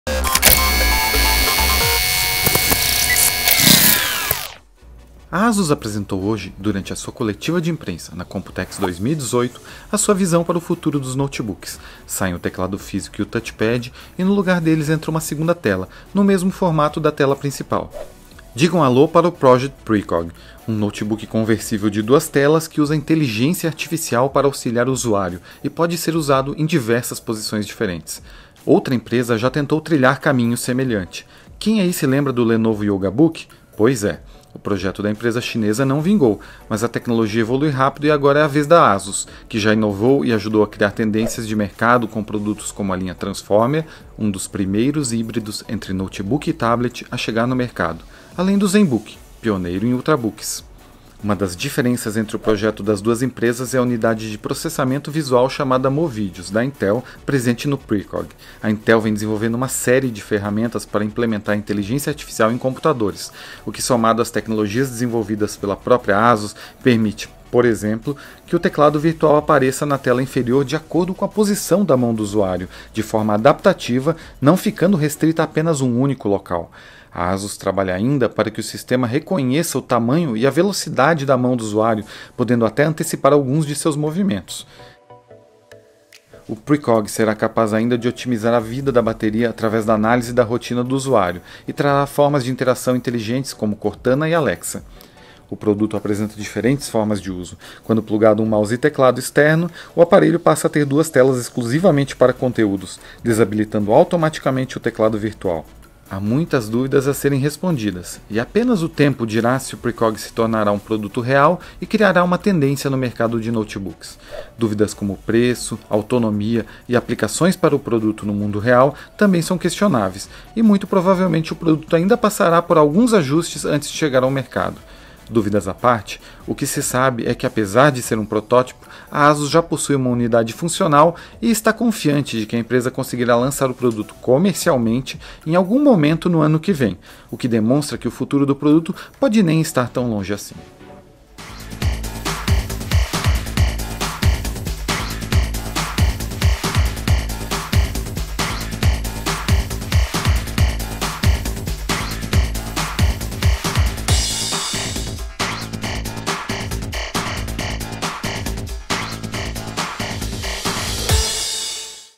Project Precog, o notebook com uma segunda tela ao invés de um teclado
A Asus não se limitou a formatos tradicionais, e arriscou alguns produtos muito diferentes do restante do mercado. o Projeto Precog é um dos mais ousados entre esses designs da empresa: ao invés de um teclado, o notebook traz uma segunda tela na parte inferior, que usa inteligência artificial para saber de forma automatizada o que deve exibir, seja a posição do teclado ou até mesmo antecipar comandos do usuário.

O aparelho tem um formato híbrido, então pode ser posicionado de forma tradicional ou pode ser rotacionado para ser usado em modo tenda ou até mesmo como duas telas, caso você conecte um teclado e um mouse nele.

O aparelho ainda é um conceito, sem data de lançamento muito menos preços definidos. A estimativa é que esse aparelho pode chegar ao mercado em 2019.